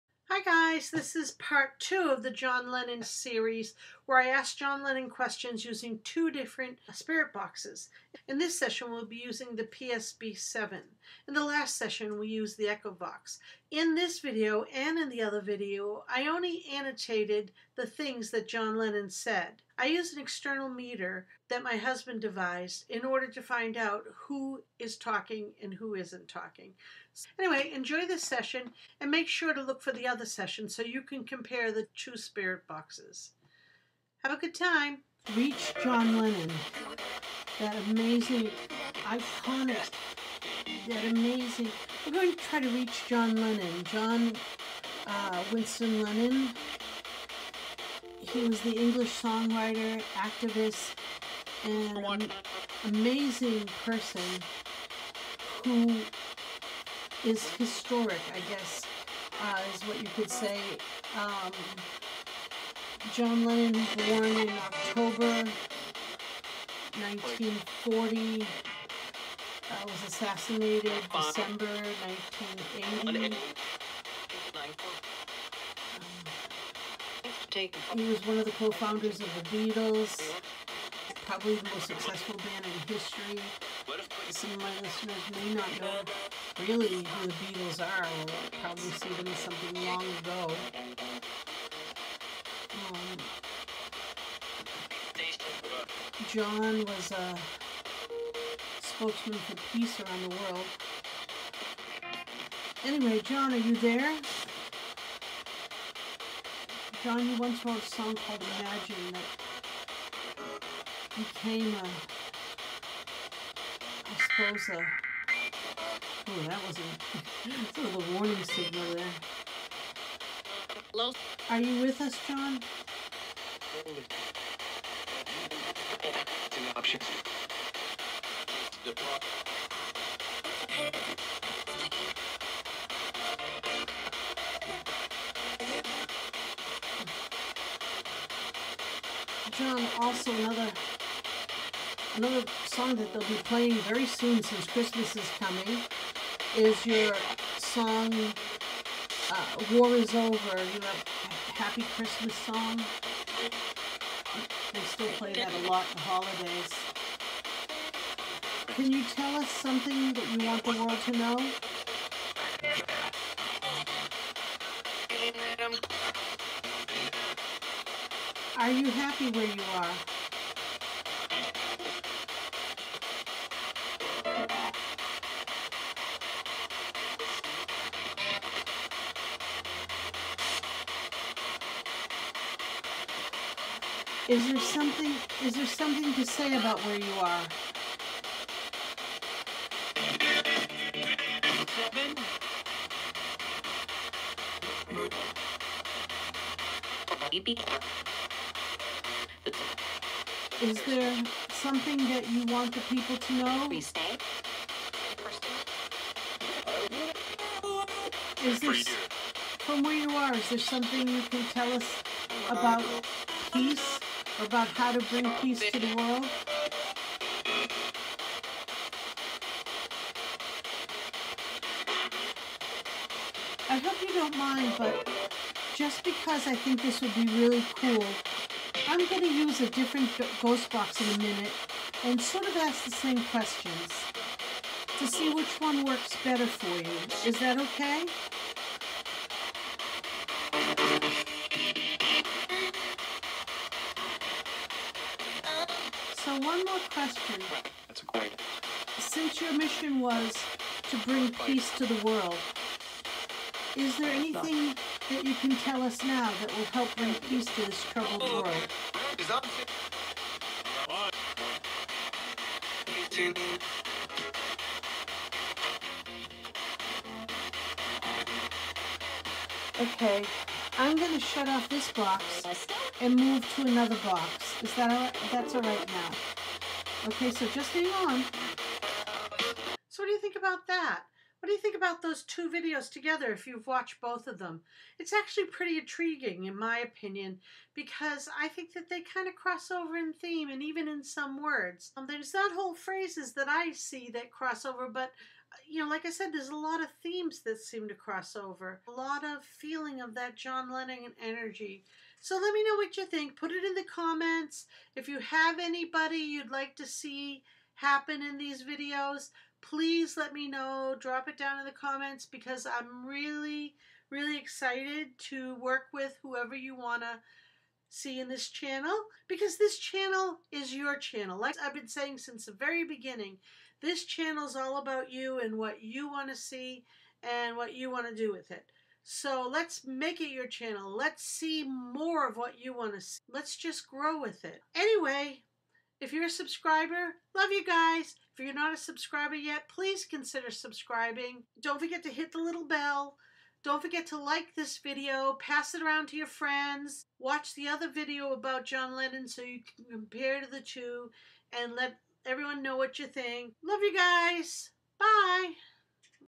JOHN LENNON Speaks from the GRAVE * The OTHERSIDE P-SB 7 Session * SPIRIT Box TEST P-SB7 vs ECHOVOX
JOHN LENNON one of the founding members OF THE BEATLES also had an outstanding solo career and was a political activist who went around the world talking about Love, Peace and the ending of war
He co-wrote many of the Beatles most popular songs along with Paul McCartney. He also wrote some of them himself.
He was a prolific and popular solo artist both in music and in art. Maybe of his solo songs are worldwide hits as well.
His legacy goes on long after his death in December 1980 when he brutally was gunned down by  Mark David Chapman in front of his home in New York City
But he returns to talk with us in this fascinating and intriguing spirit box session with the P-SB7 In the first of a 2 part series, talking to John Lennon with 2 different Spirit boxes on the same day.

Spirit Box Adventures
Unusual Things
20A Northwest Blvd